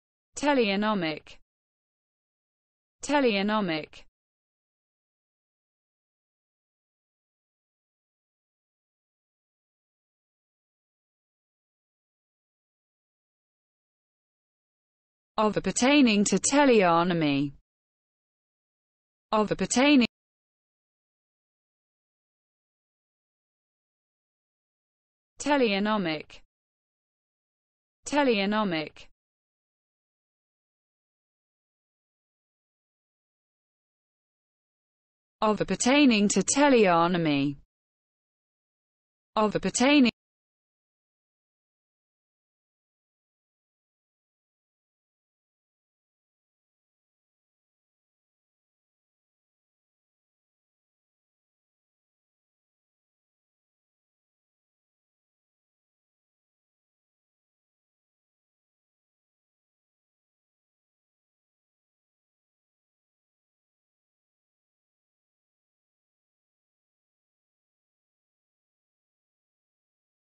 What does Teleonomic mean?
What does Teleonomic mean in English? Learn the meaning of the word Teleonomic! How to Pronounce Teleonomic. Expand your vocabulary, learn words.